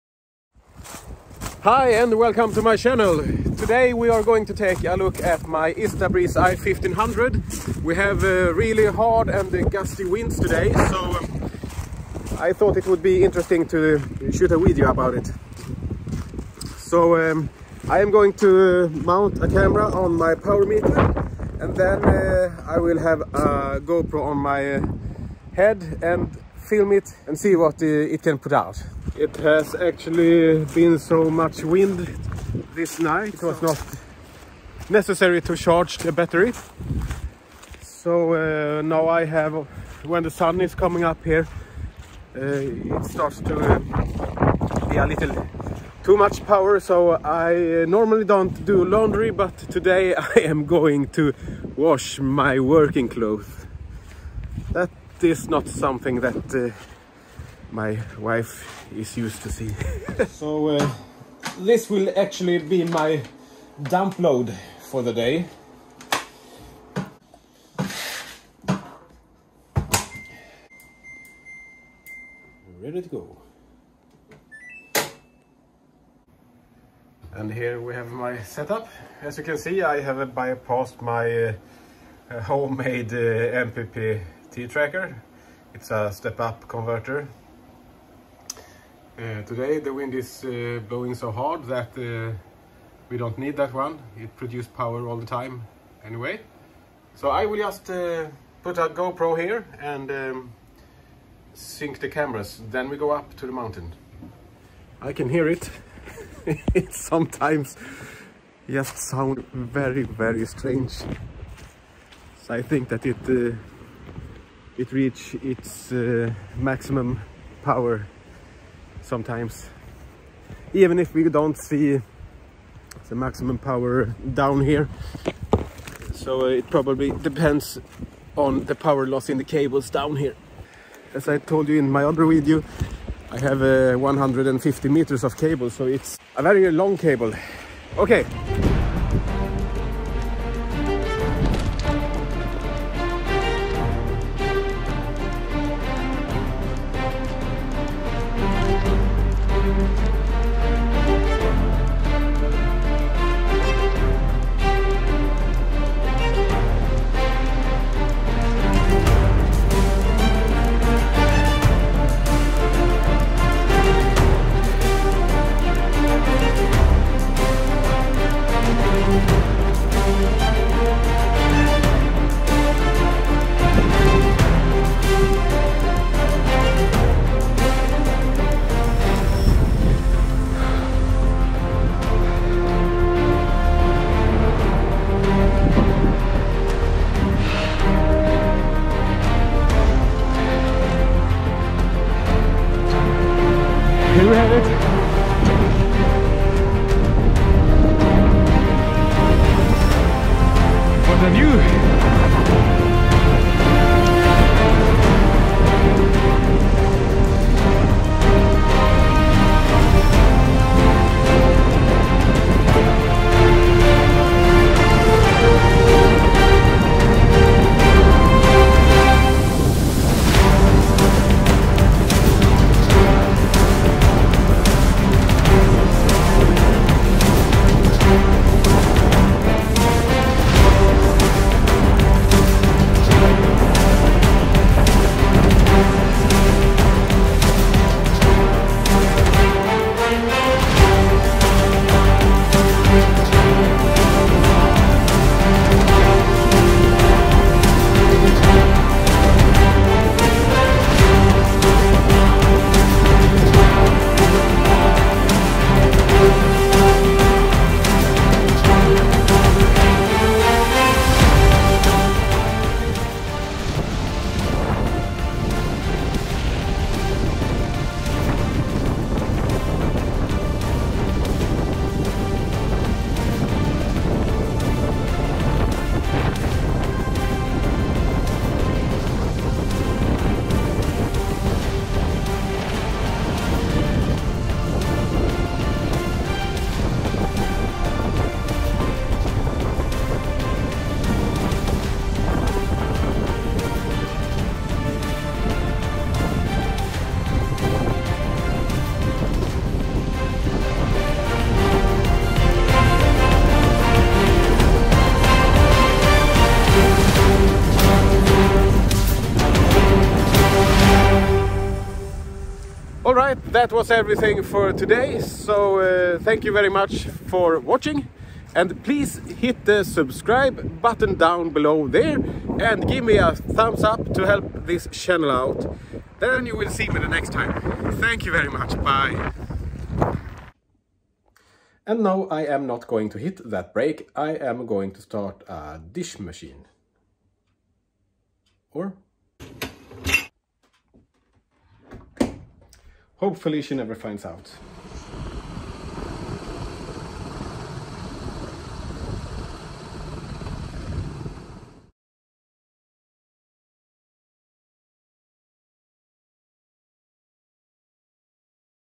Max power Istabreeze i1500 48V wind turbine in crazy 22-45 miles/h wind
Winds from 22-45 miles/h (10 to 20 m/s) Makes the windturbine sounds very strange some times in the gusts. 150 meters  of cable (500 feet) 3 phase 4mm2 (11awg)
resoults in 300watt of powerloss at 26A and 53 volts so max power indicated
1375W plus 300w loss in my cable = 1675W in total!